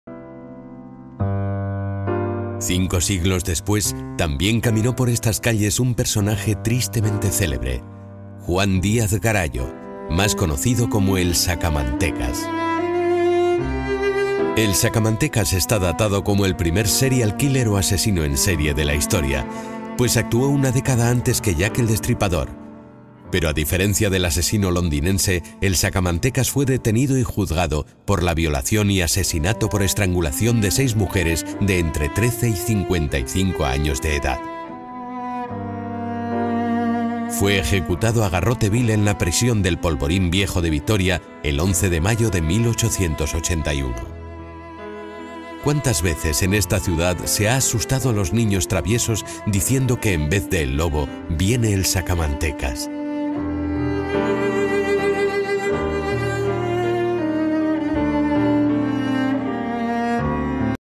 Gasteiz padeció al primer asesino en serie de la historia
Juan Díaz de Garayo, conocido como el Sacamantescas, fue el primer asesino en serie conocido de la historia, pues actuó una década antes que Jack el Destripador.